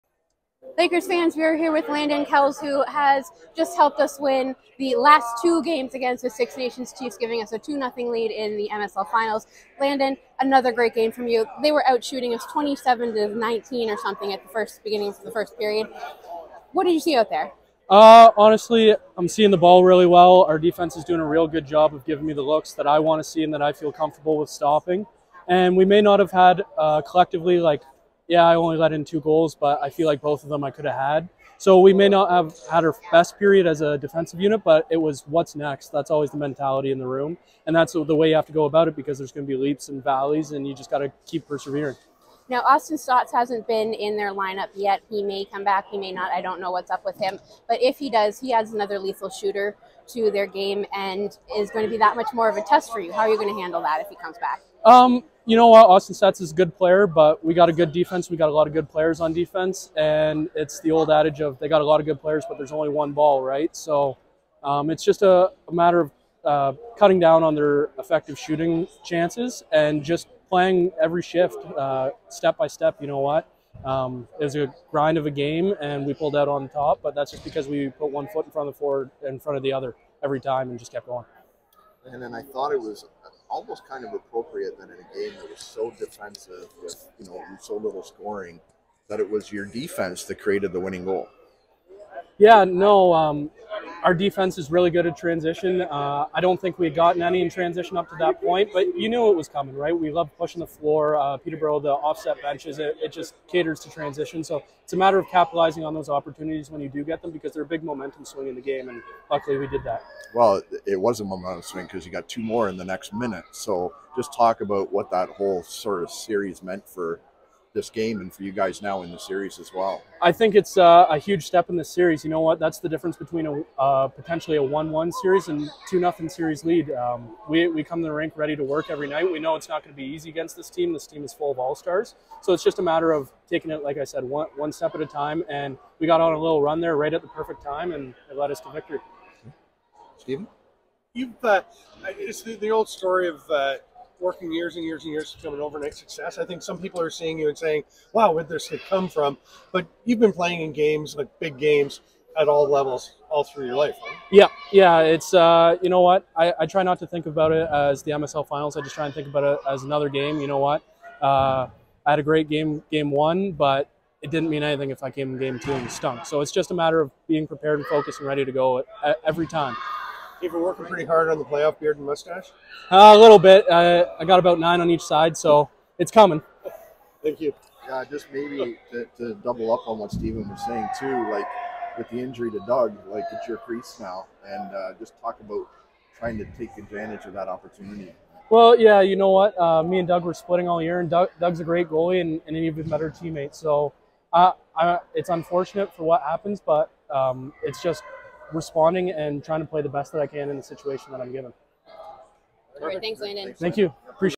Landon Kells | Post-Game August 15
Goaltender Landon Kells has stepped up HUGE for the Lakers in the playoffs, especially in helping to give the Lakers a 2-0 series lead over the Six Nations in the finals!